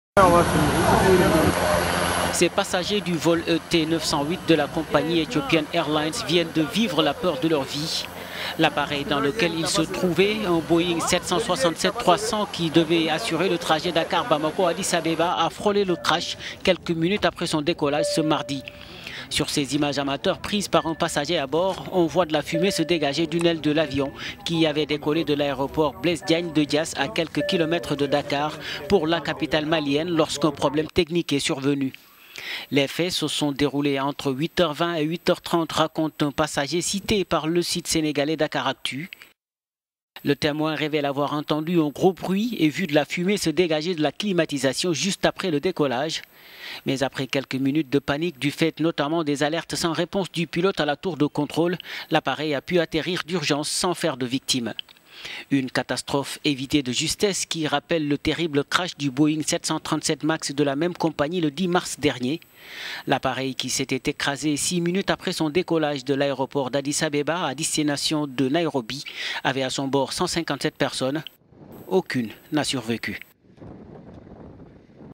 Un nouveau crash d'un vol d'Ethiopian Airlines évité de justesse
Plus de peur que de mal sur le vol ET908 d'Ethiopian Airlines reliant la capitale sénégalaise Dakar à la capitale malienne, Bamako ce mardi 8 octobre. Juste quelques minutes après son décollage de l'aéroport international Blaise Diagne près de Dakar, l'avion, un Boeing 767-300, a manqué de se crasher en raison d'un souci technique.

Un passager contacté par la rédaction du site Dakaractu explique que « dès le décollage, il y a eu un gros bruit et la climatisation a dégagé de la fumée à l’intéri…